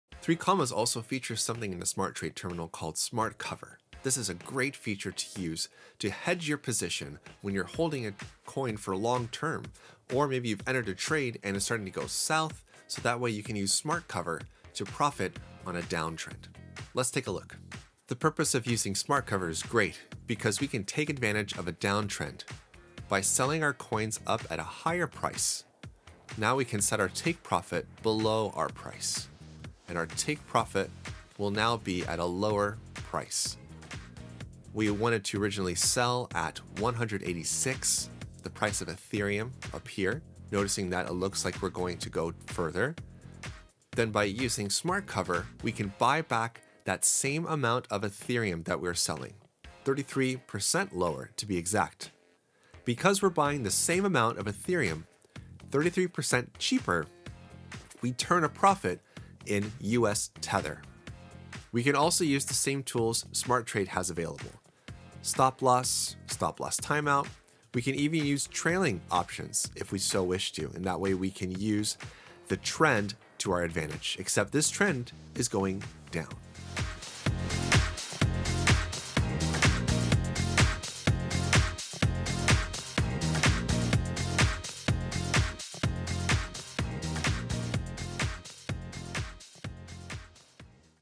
What is Smart Cover and how to use it. Trade on 3Commas.io
3Commas also features SmartTrade toll called SmartCover - a great feature to hedge your position when you holding a coin for a long term or use it to get profit even on a downtrend!

Also, 3Commas provides courses for trades. Check the 3Commas academy for novice and pro traders